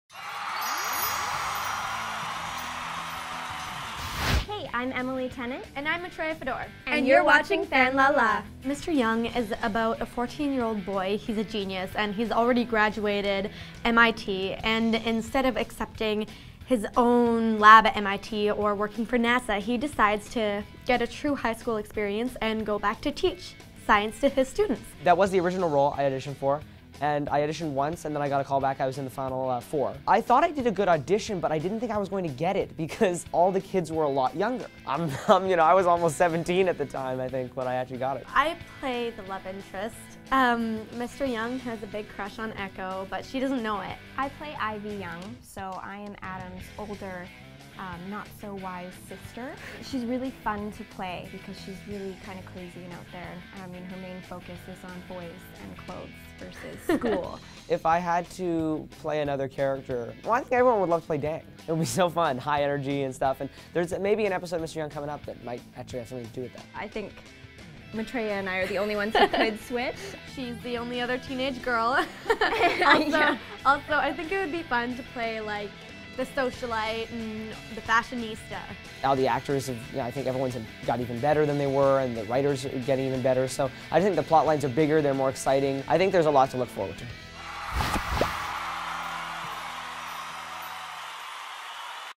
Disney XD's Mr. Young Cast Talks Cast Switch Ups and More!
Disney cutie Brendan Meyer tells us all about Mr. Young and his audition! While beauties Matreya Fedor and Emily Tennant break down who they would switch characters with and why!